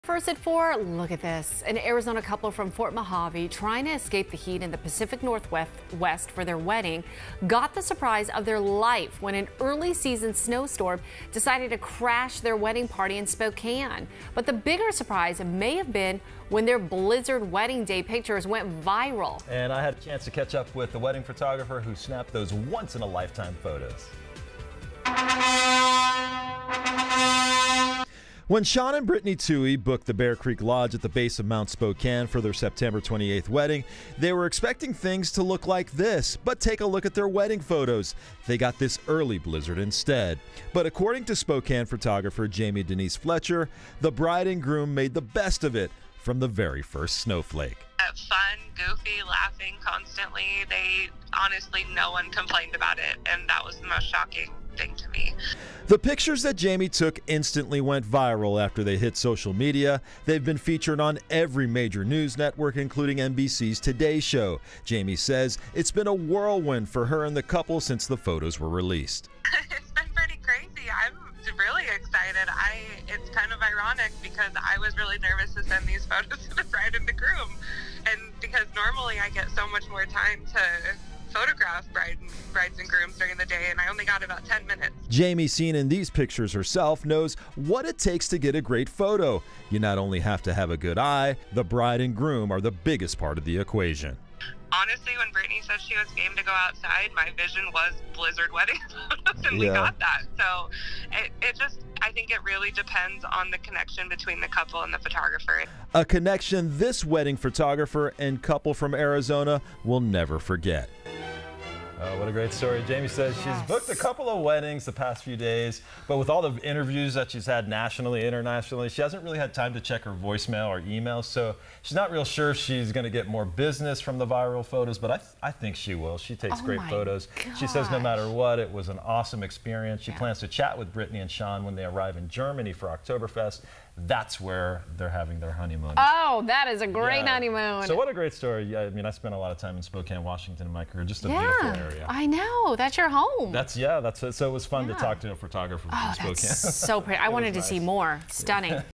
Surprise September snowstorm takes Arizona wedding couple by surprise
An Arizona couple from Fort Mohave got the surprise of their life at their September wedding in Spokane. A freaky snow storm!